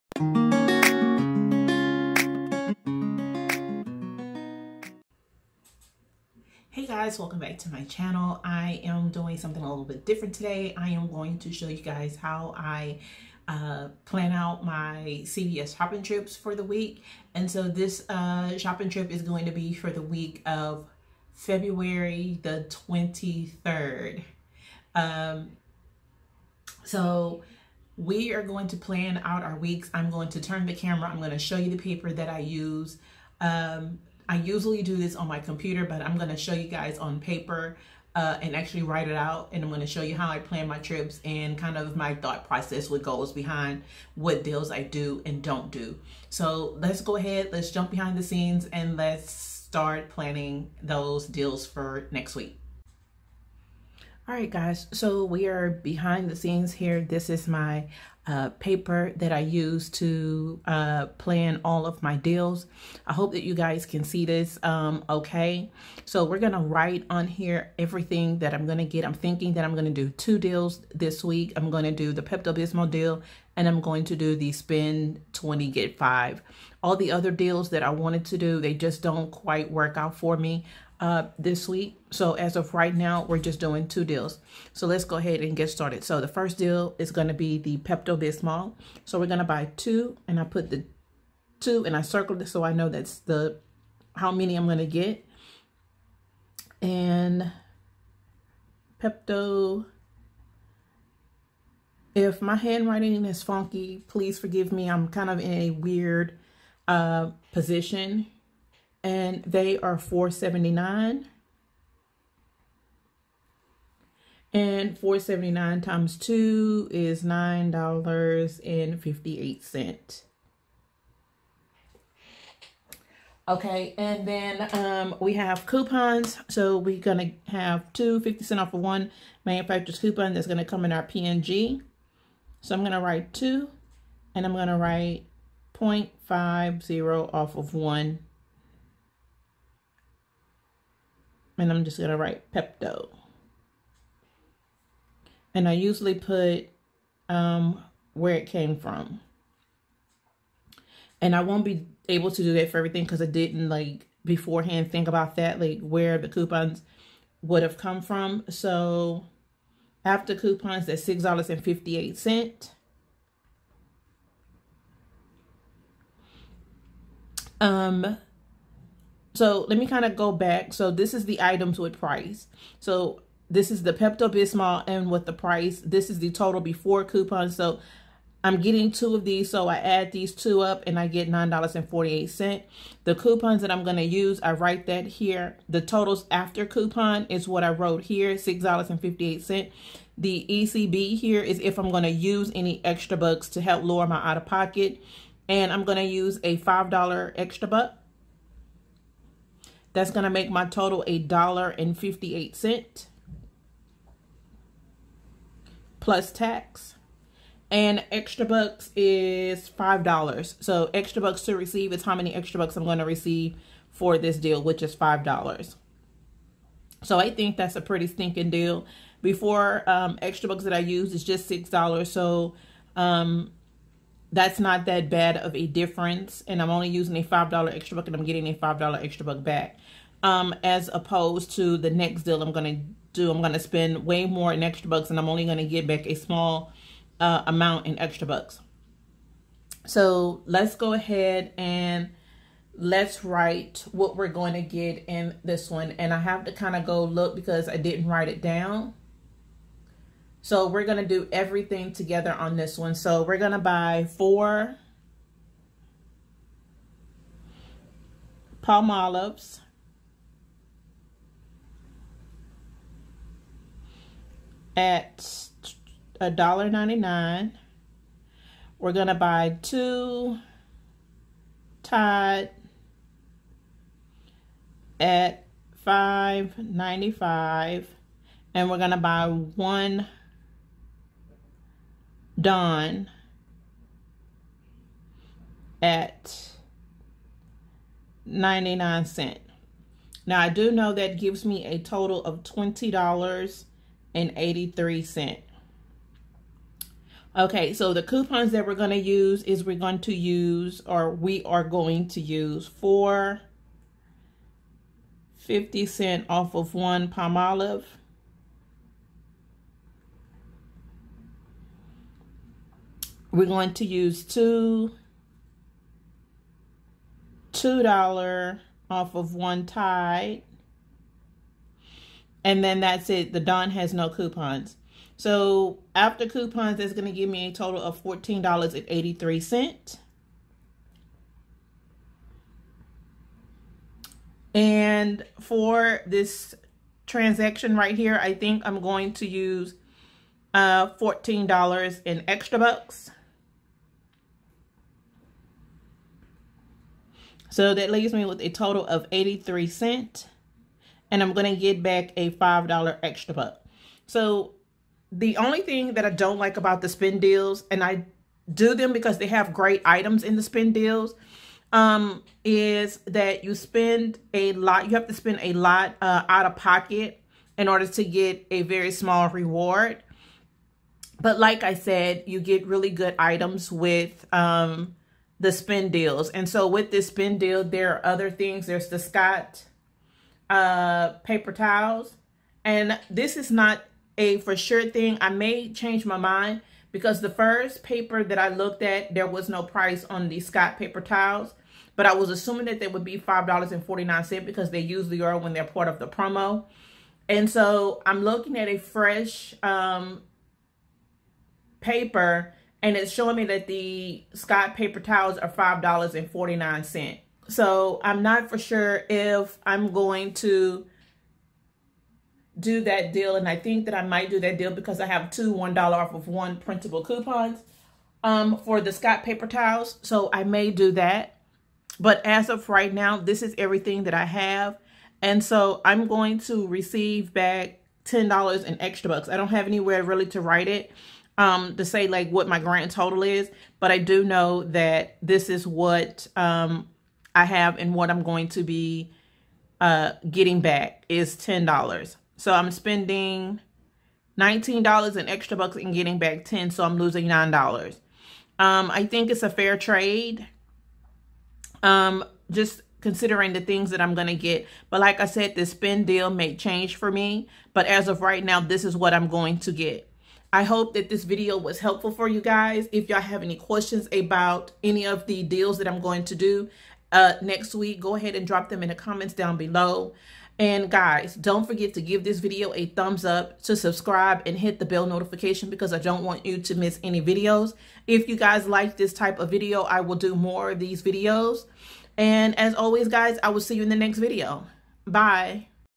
Planning My CVS Couponing Shopping Trip
Here is how I plan my couponing shopping trip at CVS each week.

Like it, if you love it! 👍 Let's get to 5,000 LIKES!

✔  W R I T E  ME
Couponin Diva

of you that supports Couponing With Free! We appreciate you!